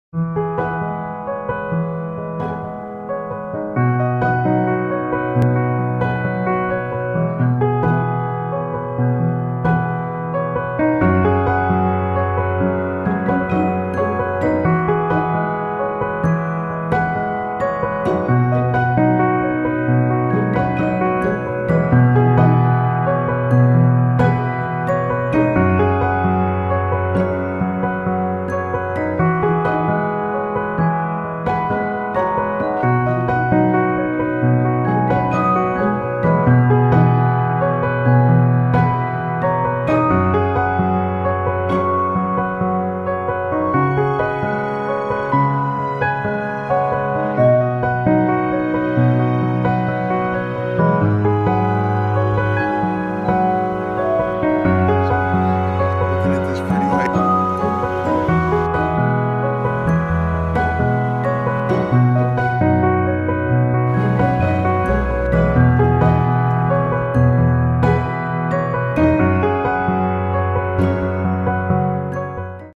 Ridenour Overlook Raystown Lake
On the last day of February me & Yvonne  made a road trip to Fly my DJI PHANTOM 4PRO DRONE at Ridenour Overlook what a wonderful way to spend the afternoon at Raystown Lake in Pennsylvania 28 February 2019